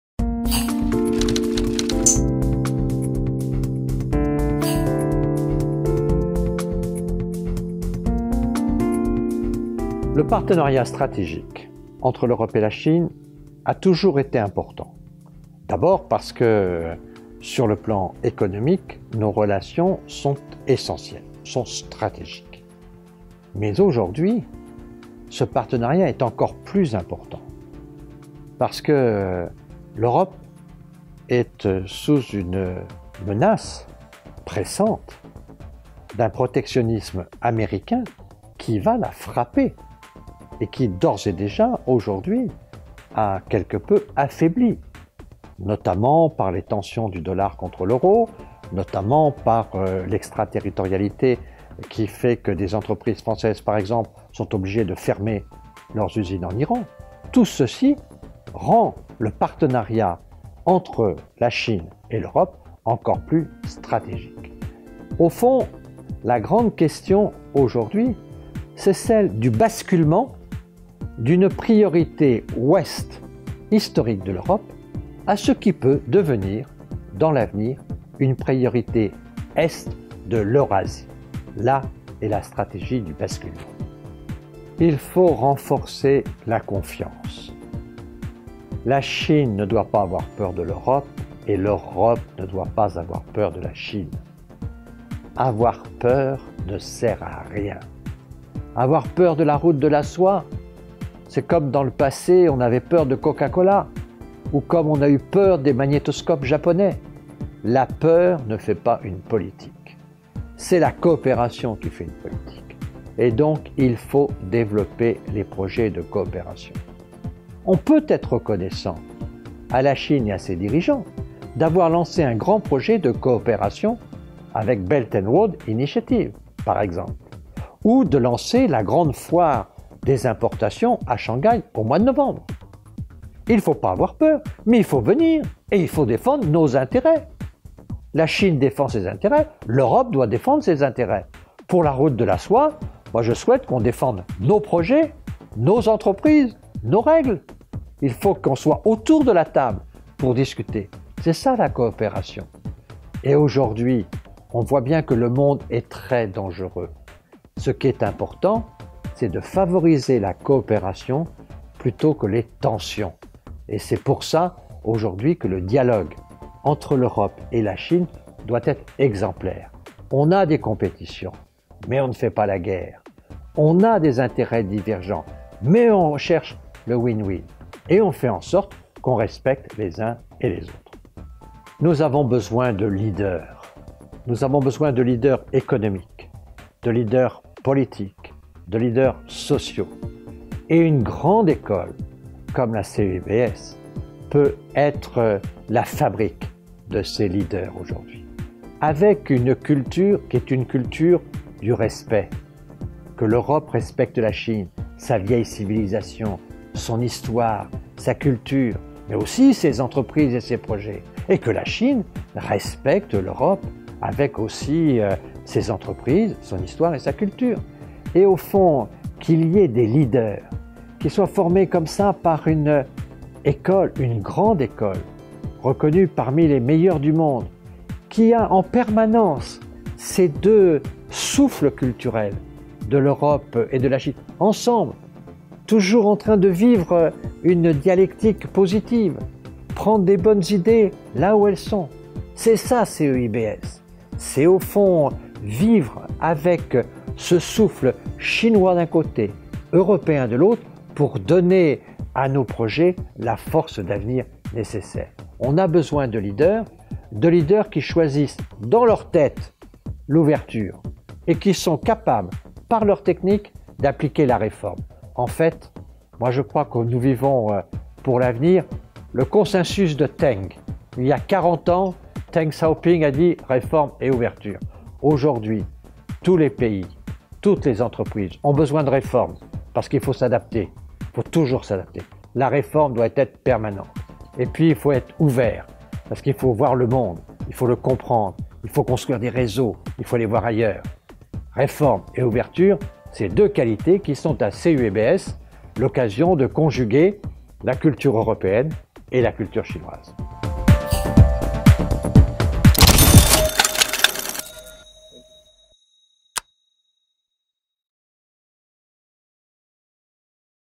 China-EU Ties: Former French PM JP Raffarin’s View
Former French Prime Minister, Charles de Gaulle Chair in Global Leadership, and Distinguished Professor at CEIBS Jean-Pierre Raffarin discusses the importance of the China-EU strategic partnership in combatting protectionism, how CEIBS is helping to produce leaders capable of embracing innovation, openness, and reform, and how cooperation between Europe and China can serve as an example for the rest of the world.